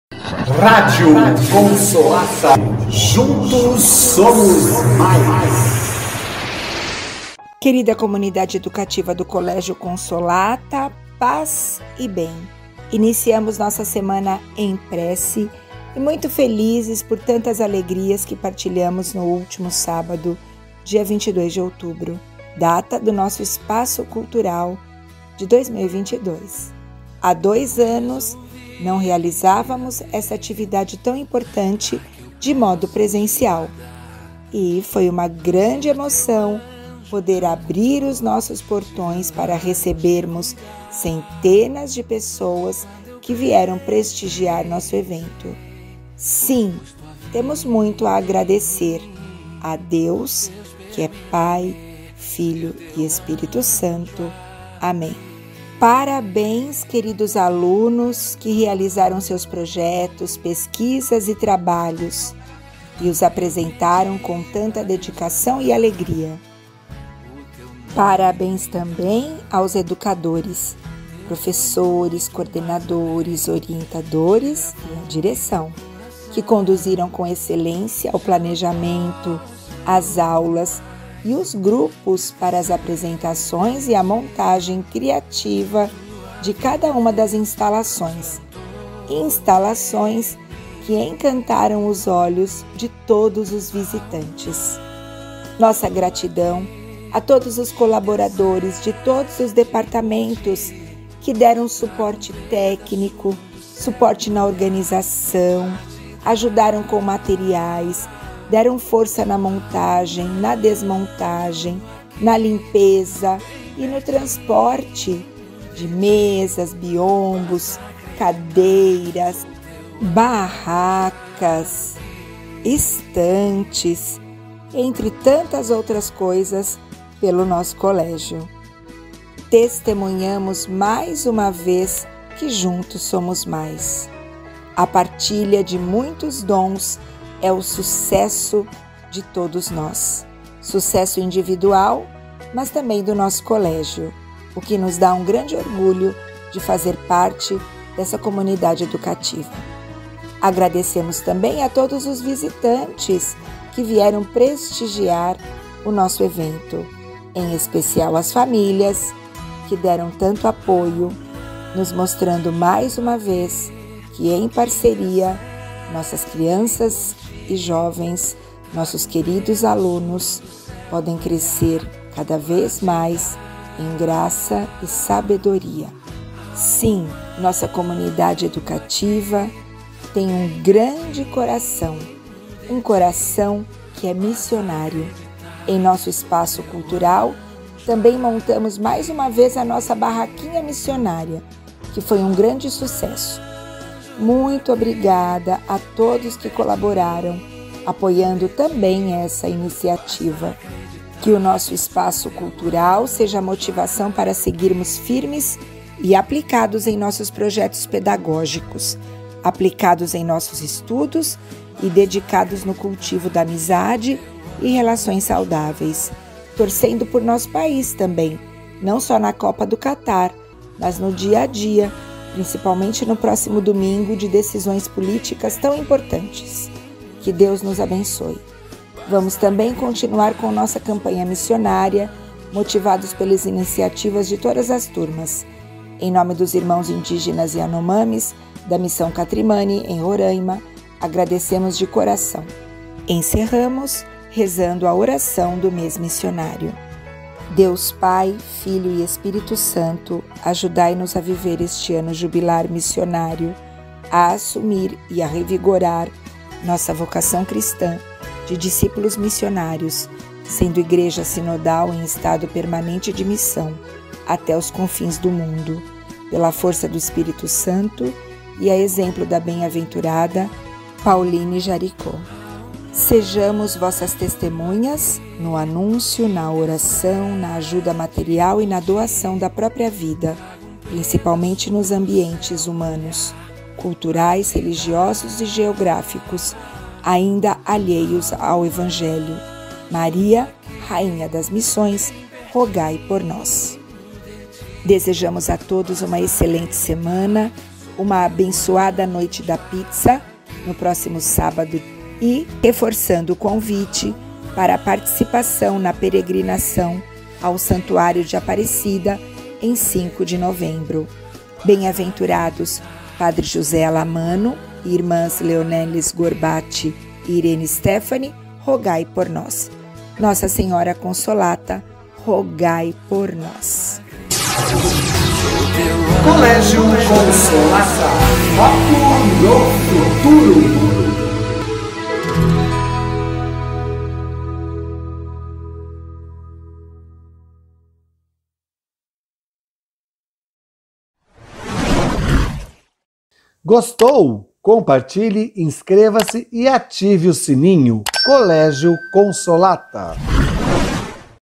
Colégio Consolata - Rádio Consolata 24/10
A mensagem da Rádio Consolata desta semana destaca o sucesso do Espaço Cultural do Colégio Consolata de 2022, um momento privilegiado de apresentação à toda comunidade educativa dos projetos interdisciplinares desenvolvidos por todas as turmas. Outro tema é a campanha missionária deste ano: “A Igreja é missão” que tem como inspiração bíblica “Sereis minhas testemunhas” (At 1,8).
A rádio foi preparada com muito carinho pela professora Fátima Manoela, coordenadora da pastoral e Ensino Fundamental  I.

Seguimos unidos e em prece pela paz no mundo.
"Uma comunidade que permanece unida realizará grandes obras."
Bem-aventurado Pe. José Allamano

Colégio Consolata
Av. Imirim, 1424 - Imirim - São Paulo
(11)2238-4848